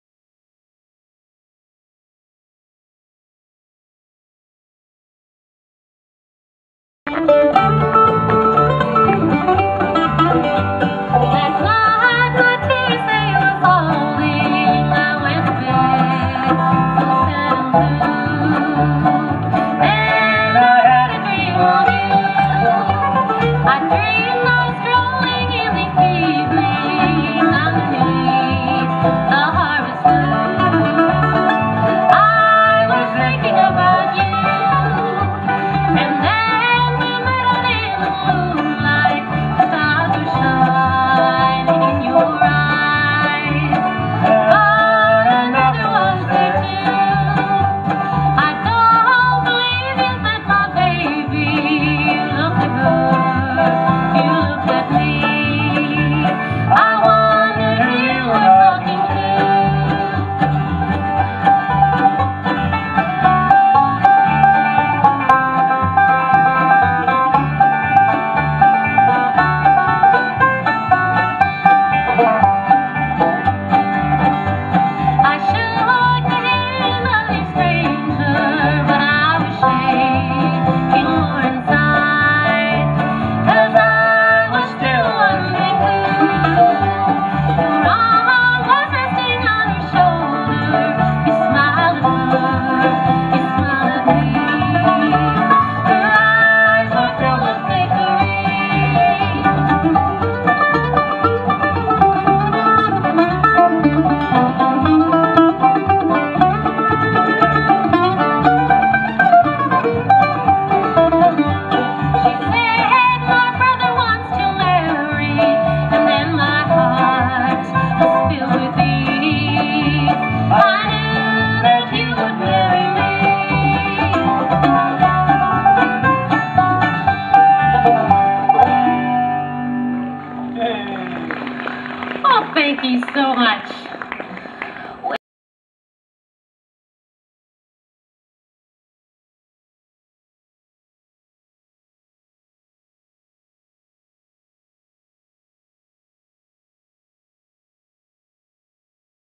Simcoe County Ramblers - Minden Hills Bluegrass Festival
The Simcoe County Ramblers are a new Ontario bluegrass band and they delighted festival goers at the Minden Hills Bluegrass Festival on July 16 & 17, 2011. This is their first full year of playing bluegrass festivals and I hope to see them around for a while. Be sure to check them out, you won't be disappointed.